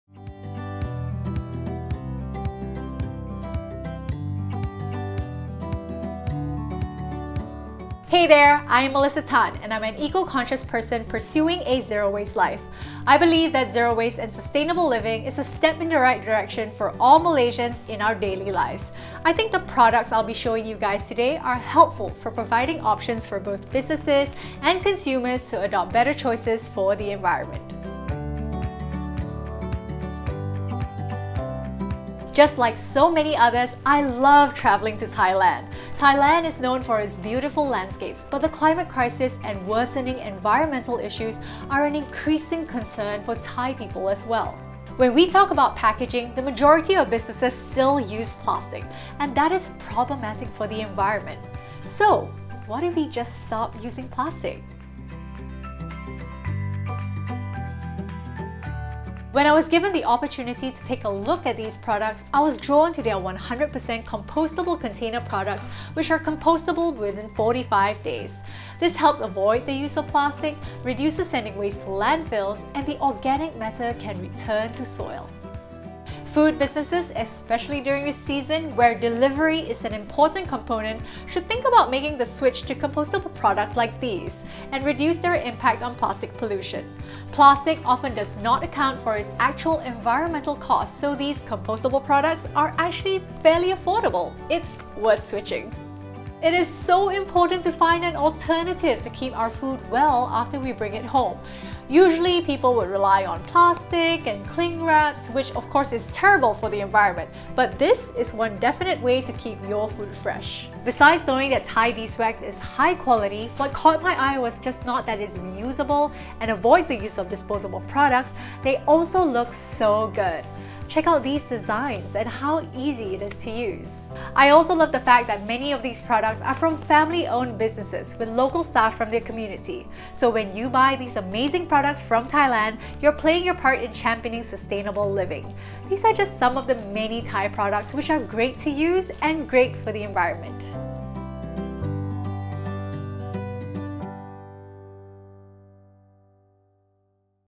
DITP Presents Eco-Friendly Packaging
Department of International Trade and Promotions ( DITP) of Thailand presents a variety of eco-friendly packaging products. In this video, the brands  highlighted Gracs and Superbee are just a few brands from a wide array of choices that are available from Thailand. These companies are export ready and will be your willing business partner.

For import or sourcing enquiries please contact